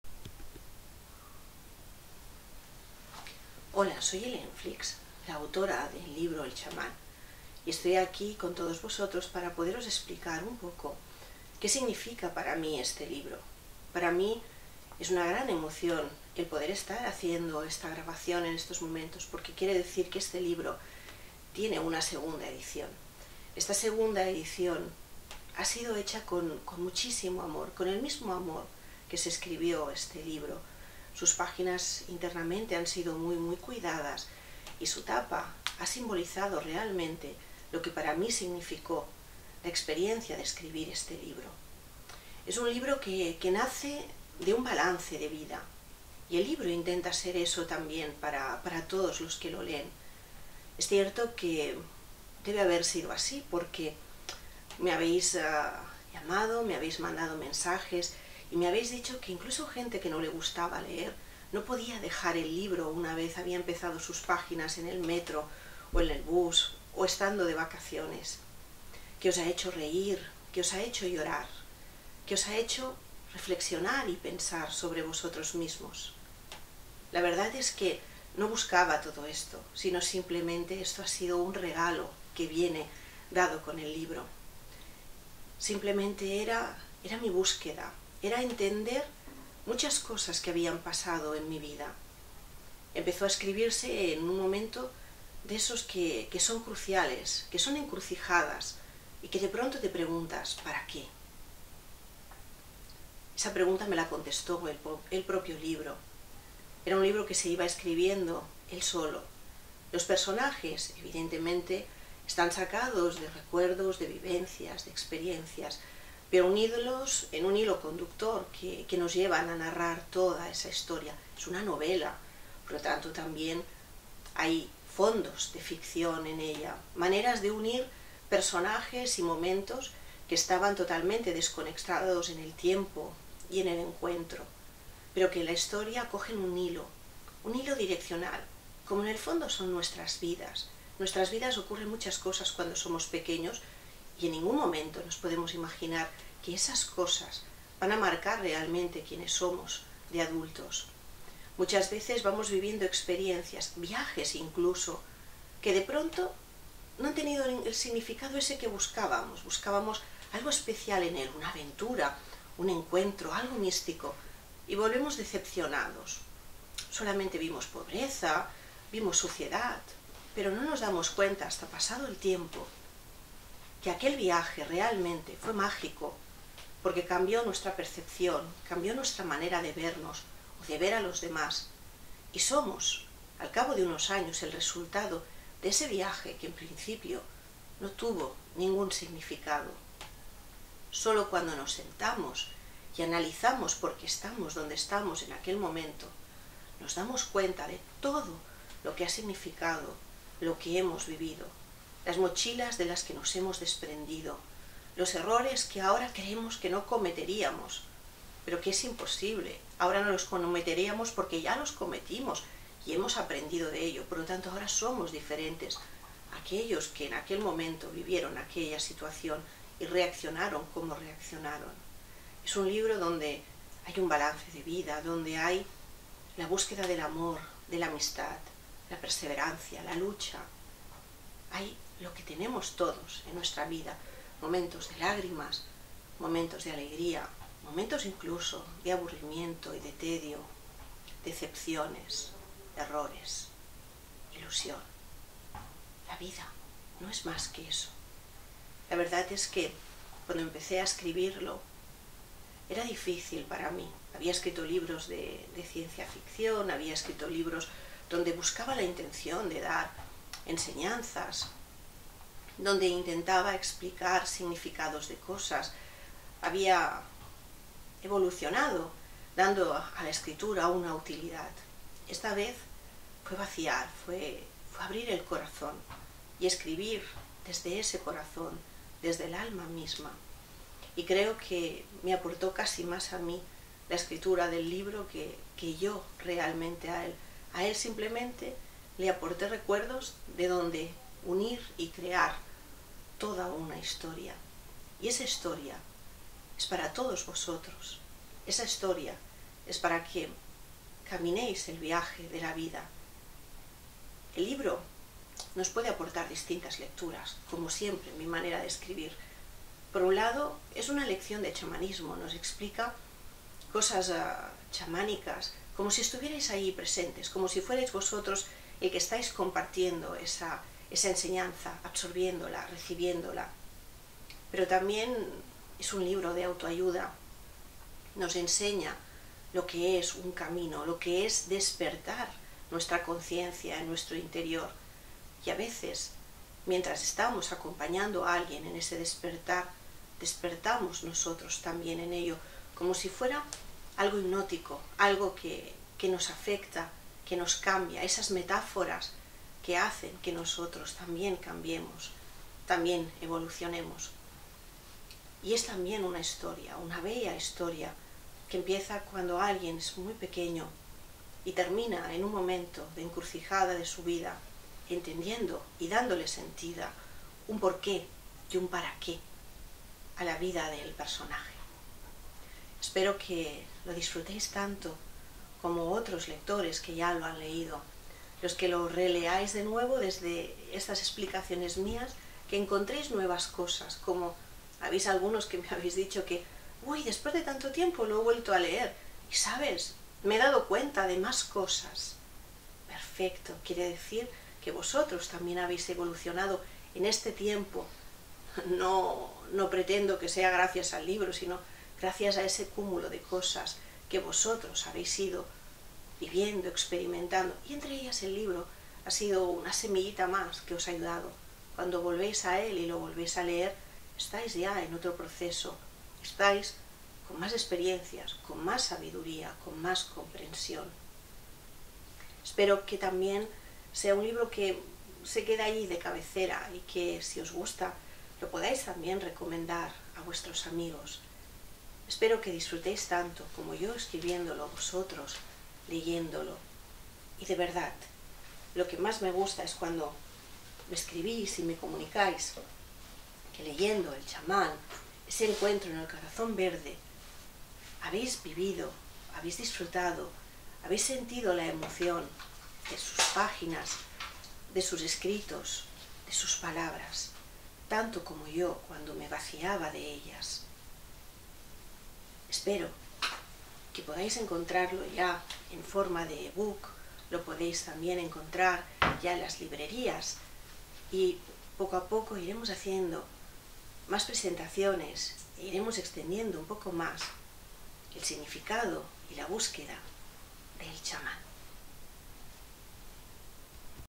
Presentación del libro "El Chamán"
Helen Flix comenta los motivos que le llevaron a escribir  "El Chamán" y lo que les puede aportar a los lectores.